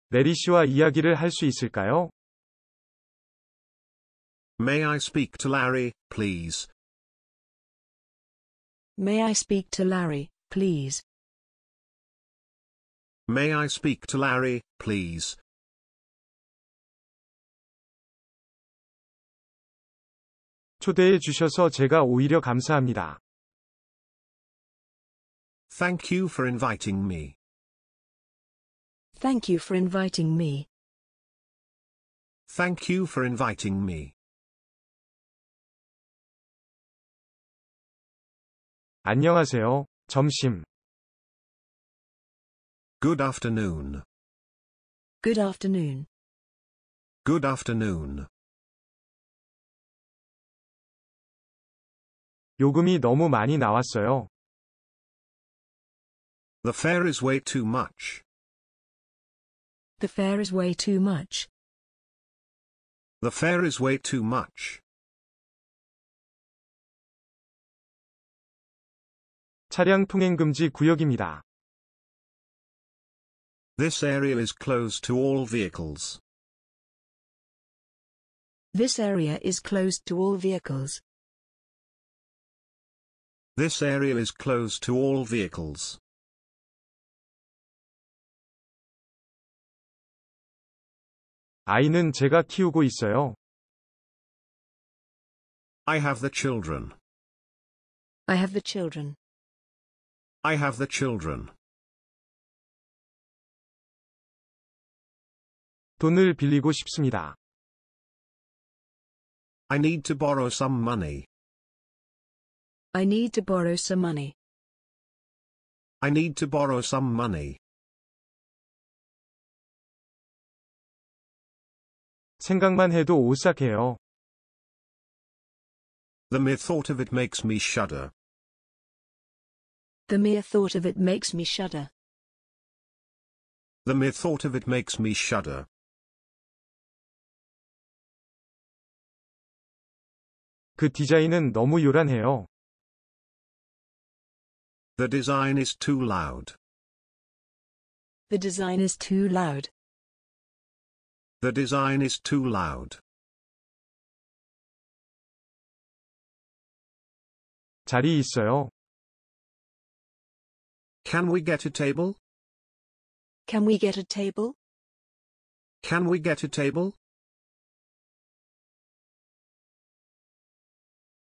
매일매일 무작위 생활영어 10문장 #110 (2020-10-13)
May I speak to Larry, please.   | 래리씨와 이야기를 할 수 있을까요?
Thank you for inviting me. | 초대해 주셔서 제가 오히려 감사합니다.
Good afternoon. | 안녕하세요. (점심)
The fare is way too much. | 요금이 너무 많이 나왔어요.
This area is "closed to all vehicles ". | 차량 통행금지 구역입니다.
I have the children. | 아이 는 제가 키우고 있어요.
I need to borrow some money. | 돈 을 빌리고 싶습니다.
The mere thought of it makes me shudder. | 생각만 해도 오싹 해요.
The design is too loud. | 그 디자인은 너무 요란 해요.
Can we get a table ? | 자리 있어요?